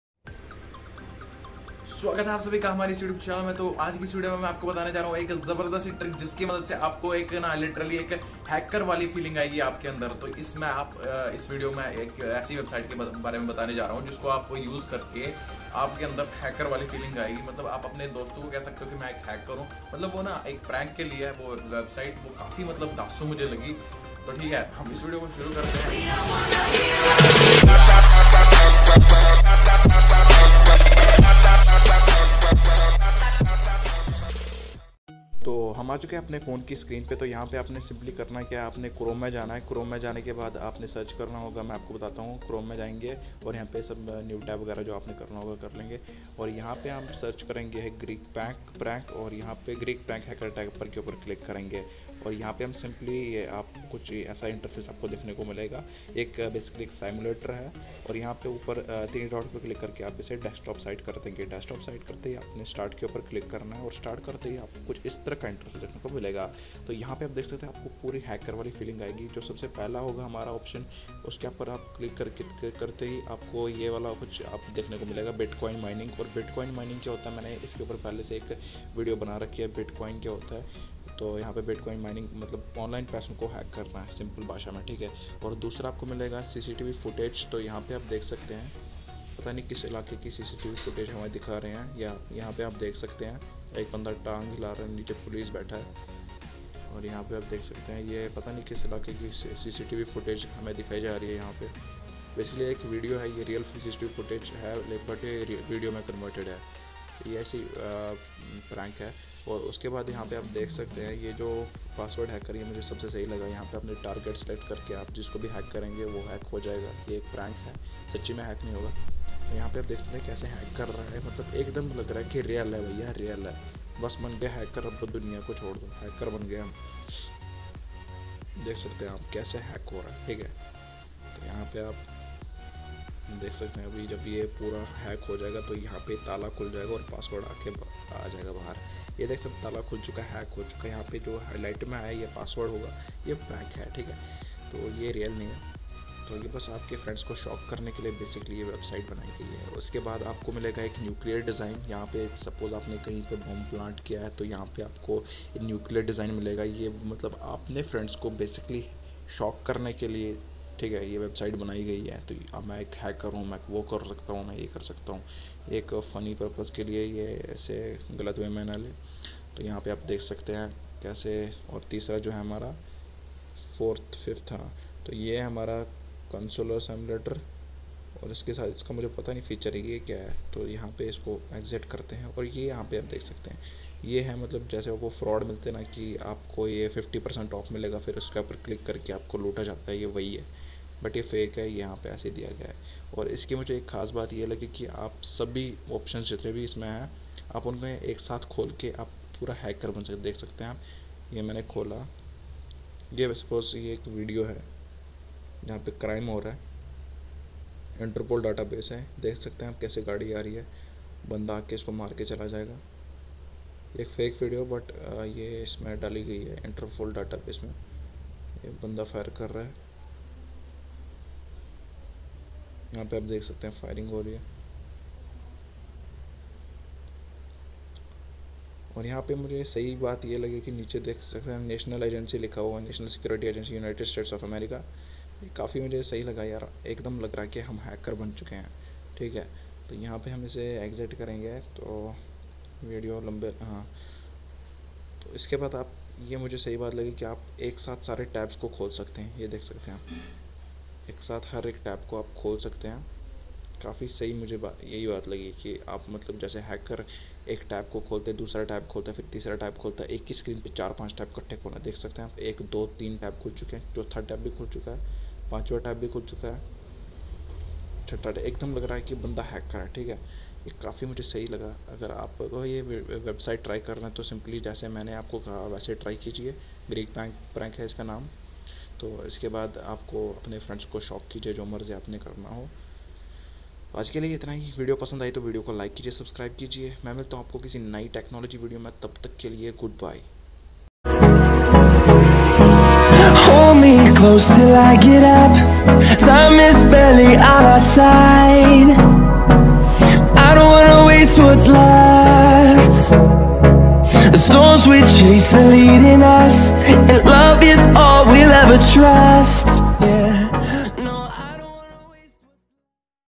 Become a Hacker in two minutes through this Amazing Website 🤟😂😂!!!(Prank)
This video is to shock your friends that u r a Hacker..